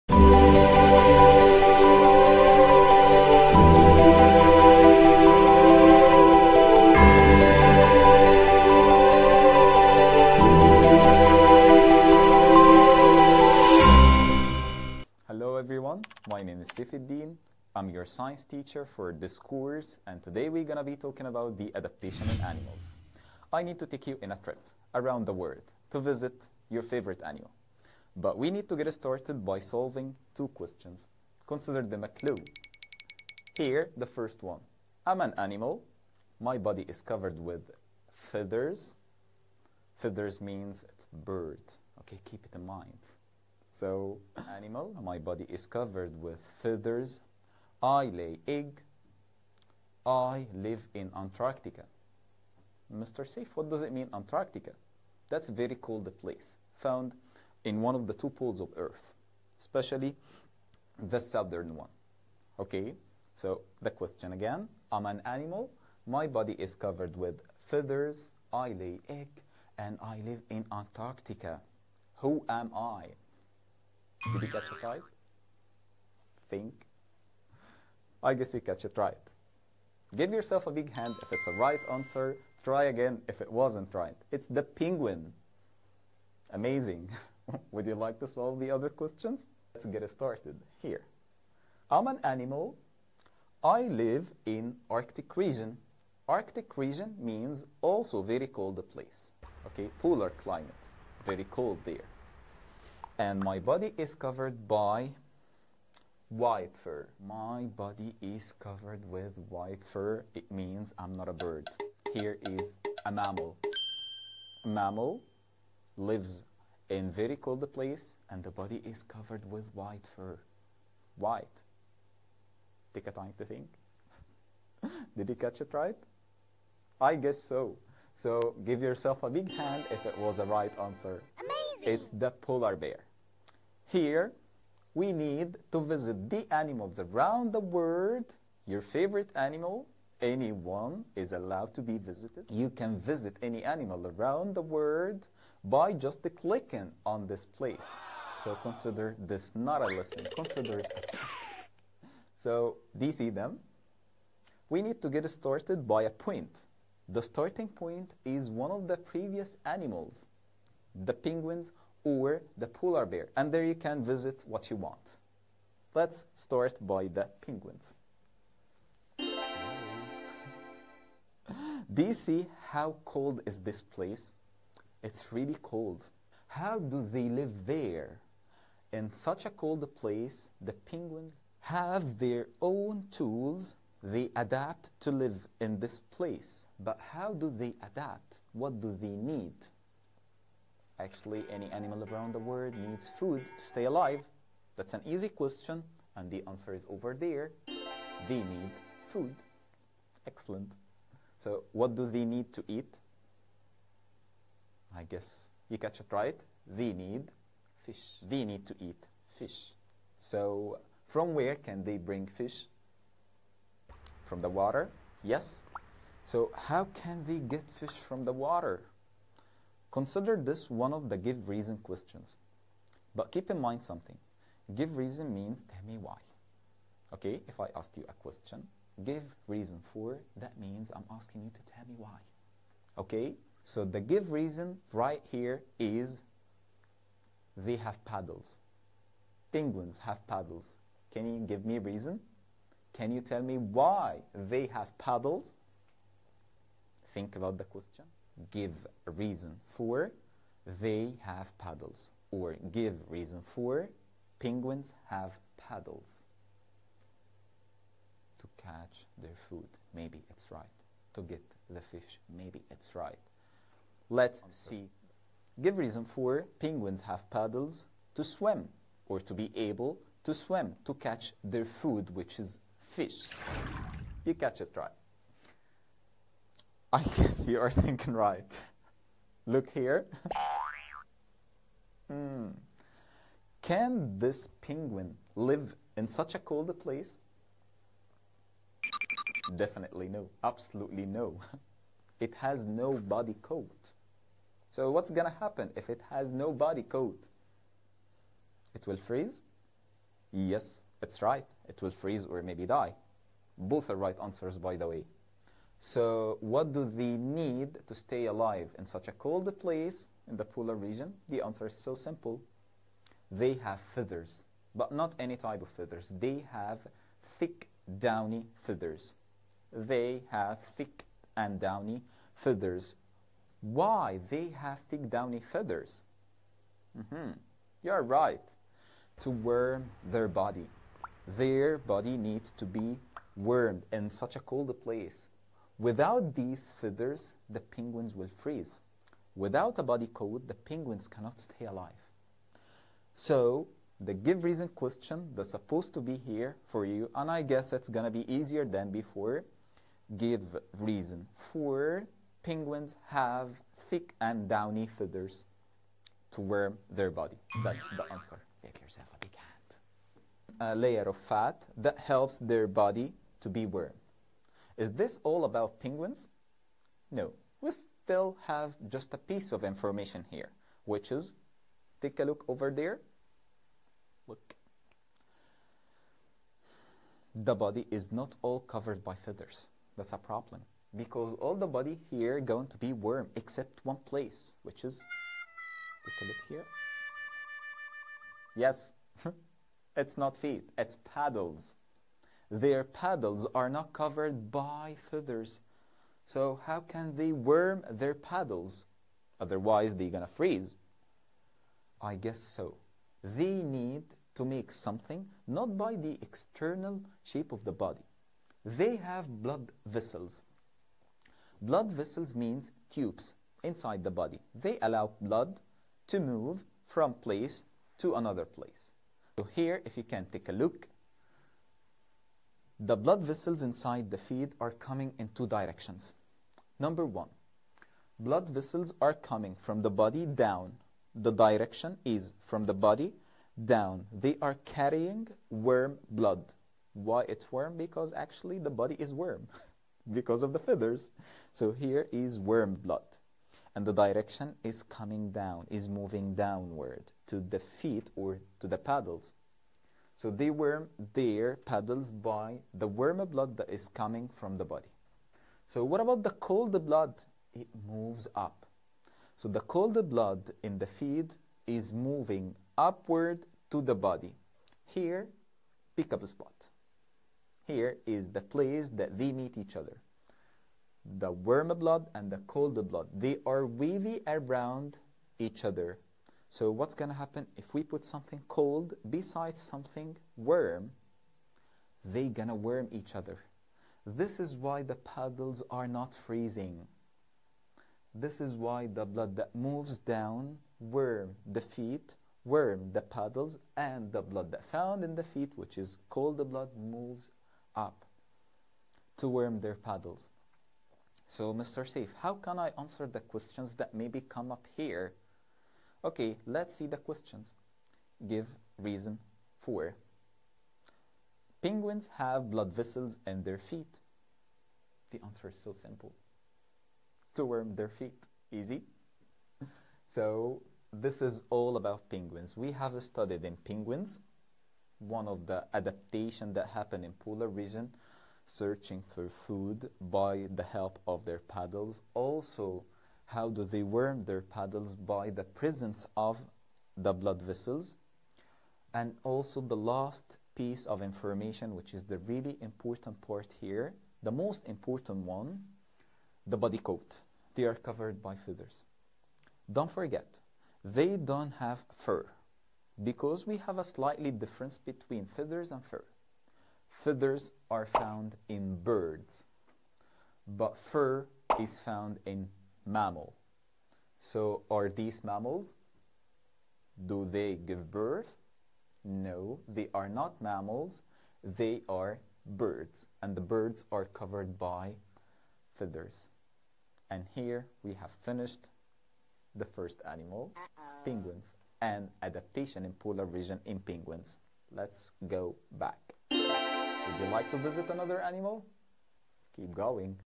Adaptation in penguin _ Science primary 4 concept 1 lesson 1 Adaptation and survival المنهج الجديد
Adaptation in penguin adaptation in animals _ Science primary 4 concept 1 lesson 1 Adaptation and survival
المنهج الجديد لمادة العلوم لغات للصف الرابع الابتدائي
المفهوم الأول الدرس الأول